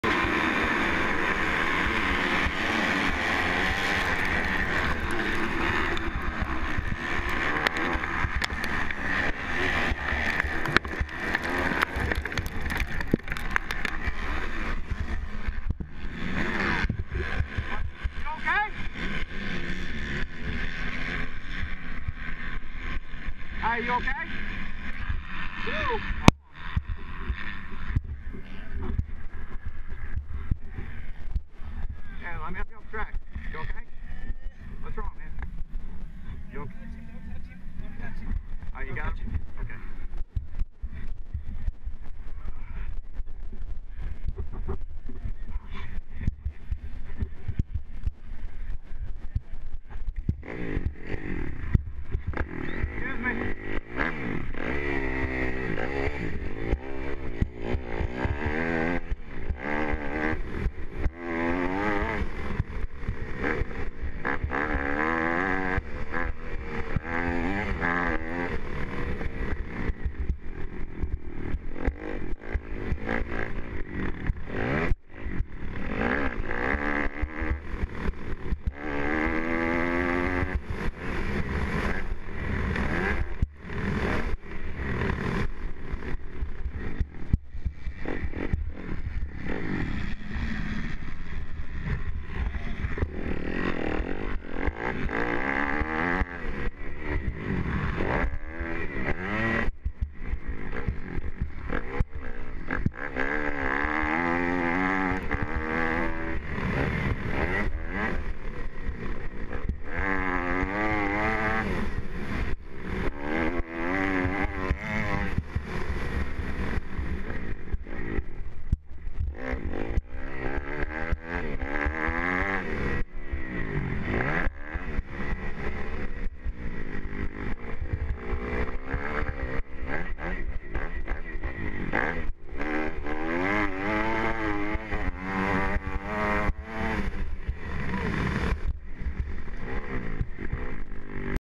BSOT final round at WTMX short version
moto 1 at wtmx for the final round of big side of texas .. 250 novice .. first lap is full of carnage